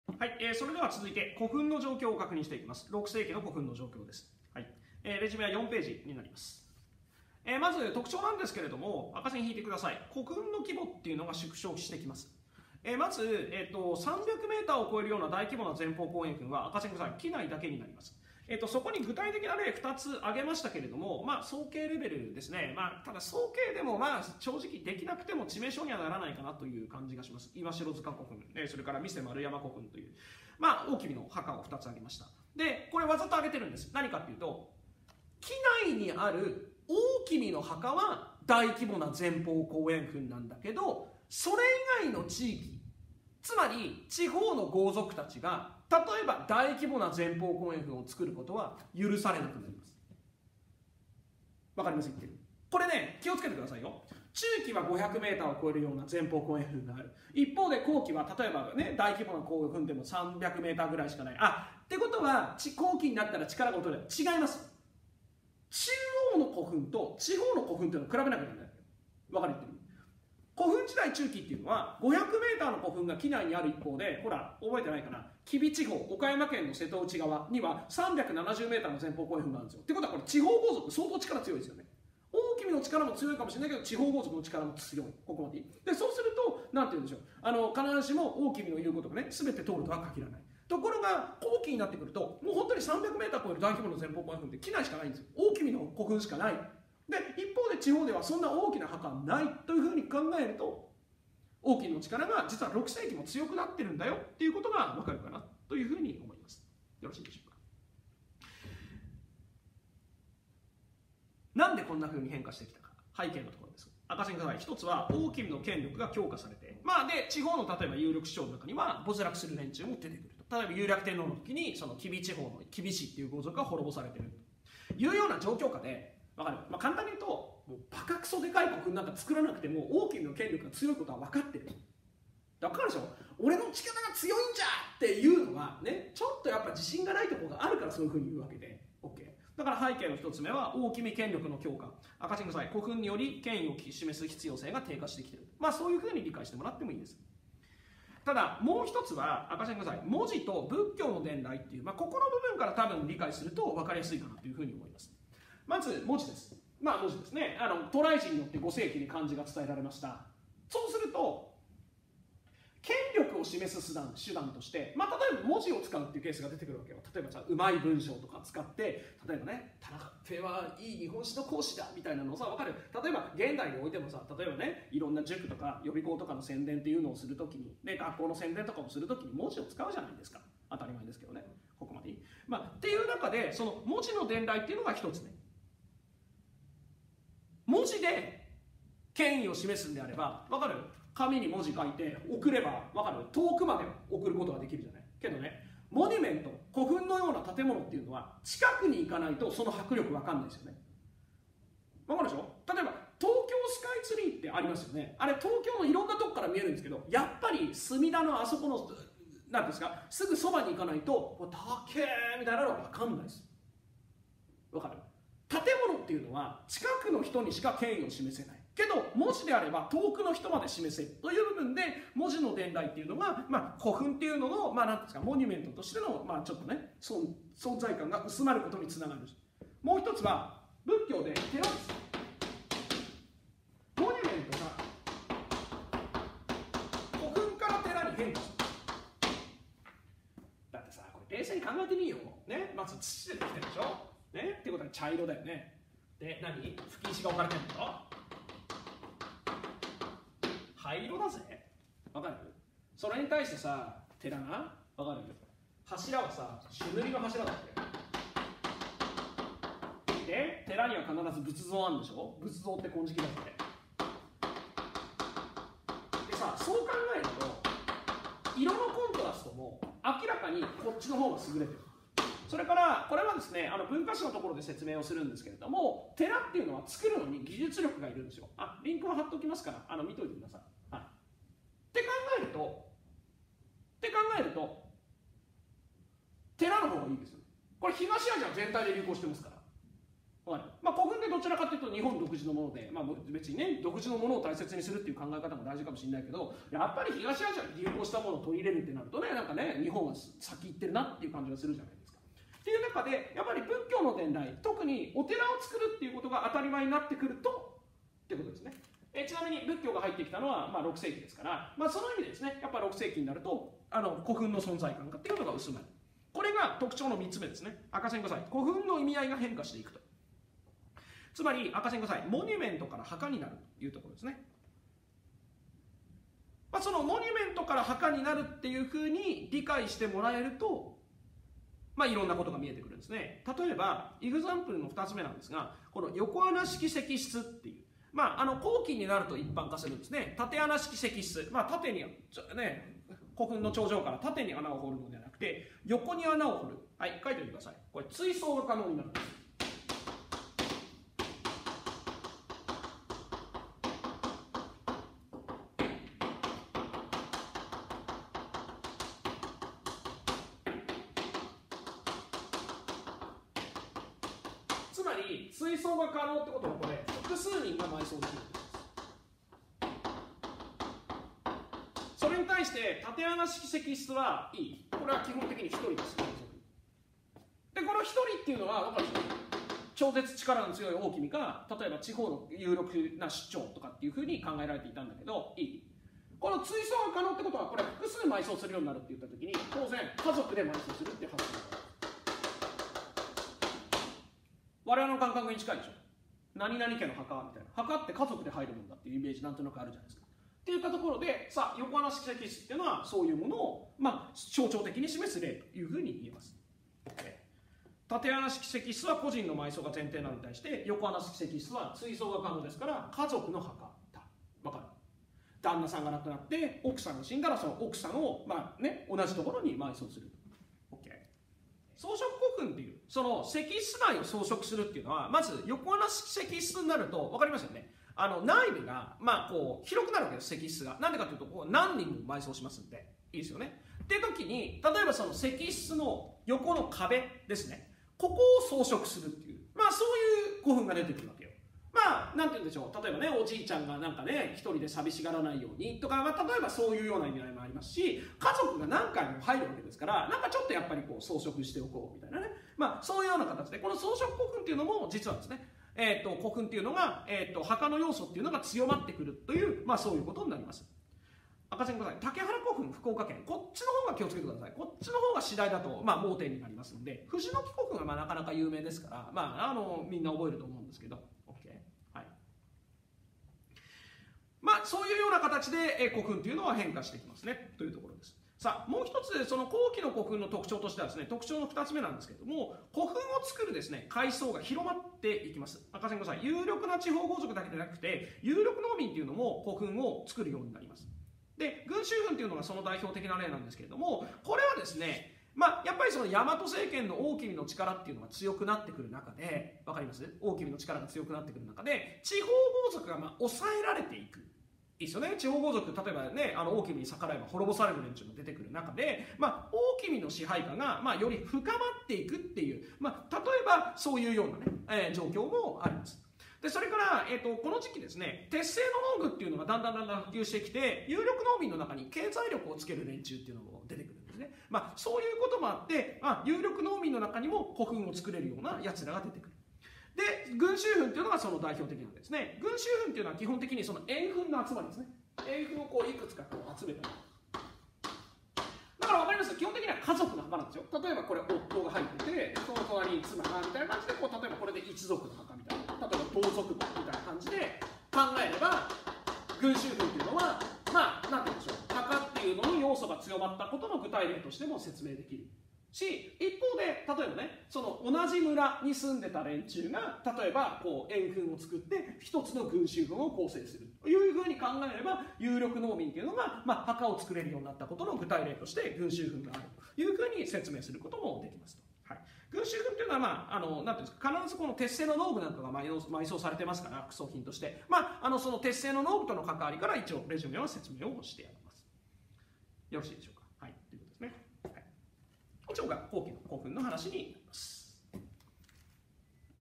古墳時代その⑫…古墳後期⑦
古墳時代後期（6世紀）の古墳の状況を確認していきます。古墳の規模は小さくなりますが、大規模な前方後円墳が畿内だけに限定されるという点から、6世紀は大王の権力が強化されていることを読み取る必要があります。単純に古墳の大きさだけで権力の推移を理解するのは誤りです。
　また、モニュメントではなく墓の要素が強まっていくのも後期古墳の特徴です。それらを解説しています。